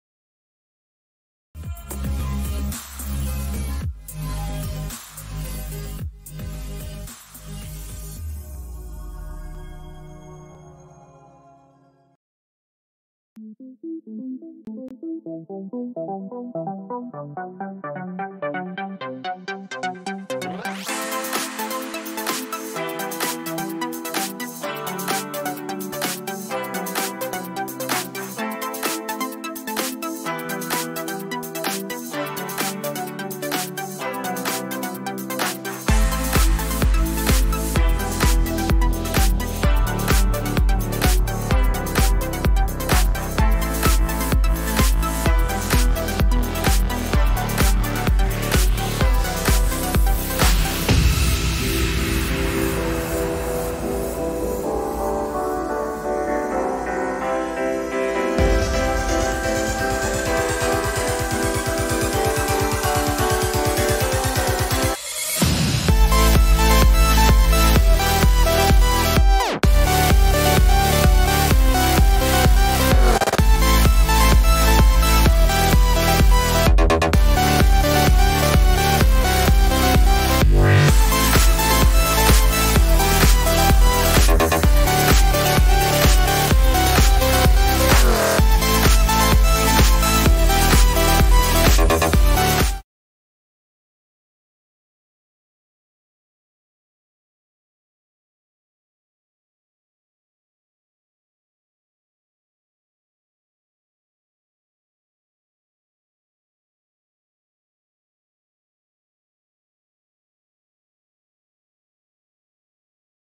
BTCUSD | Crypto Technical Analysis & Trading Idea for 2nd November 2022 by CYNS on Forex
Hello Traders,
⭕ In this video includes - ( BTCUSD Crypto Market Update for 2 November 2022 )
    ✅- Technical analysis on BTCUSD chart
    ✅- What is the next Opportunity on BTCUSD Market